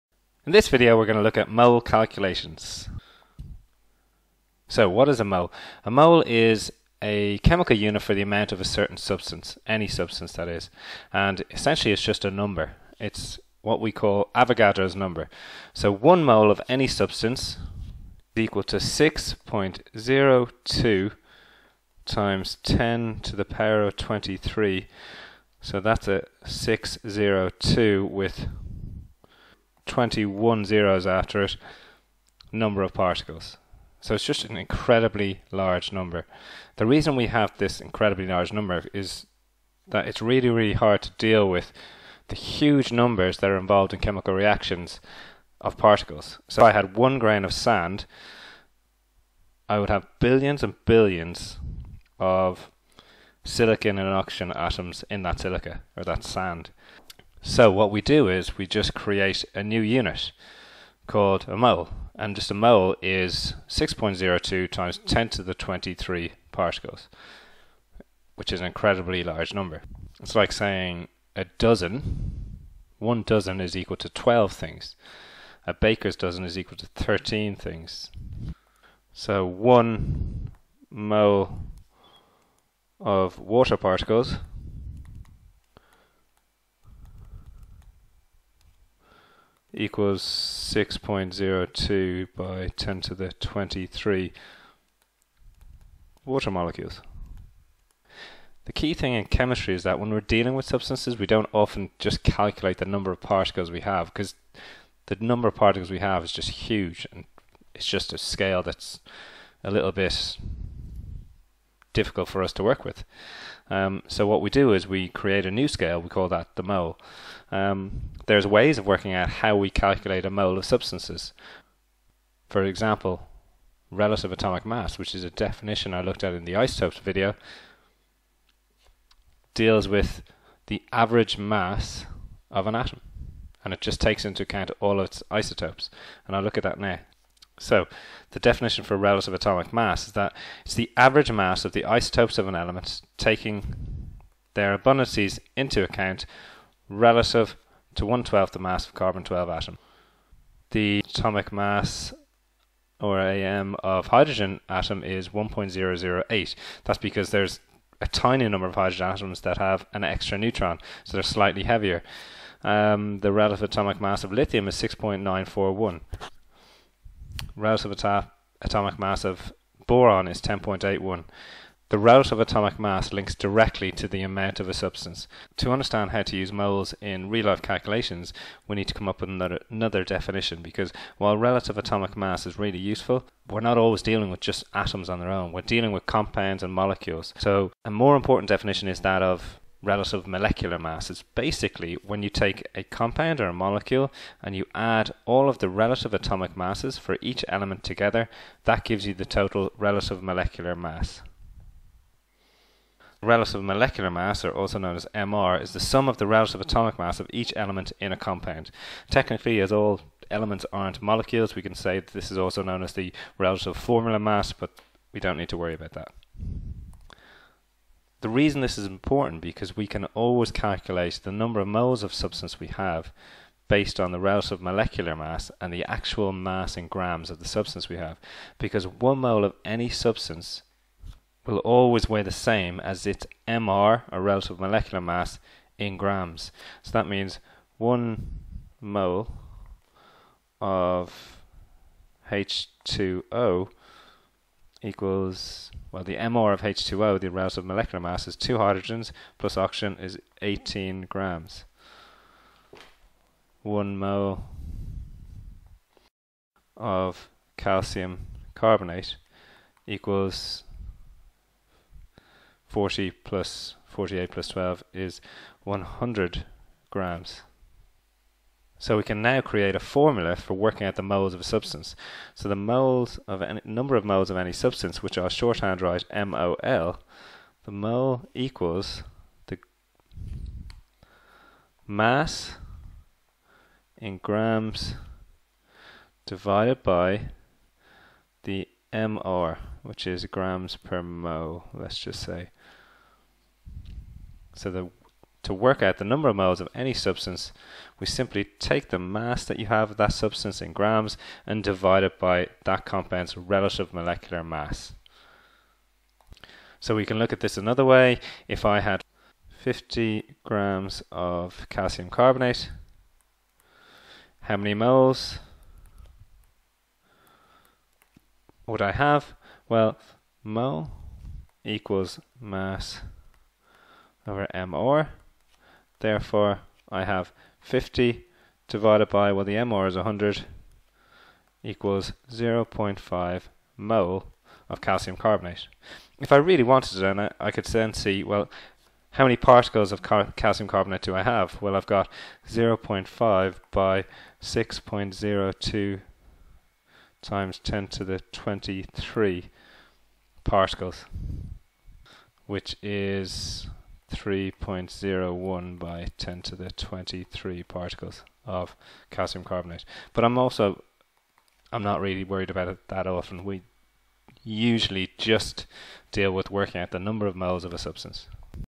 Introduction to moles calculations
A very brief introduction to the concept of a mole, being the chemical unit for the amount of a substance. Only mass calculations are considered.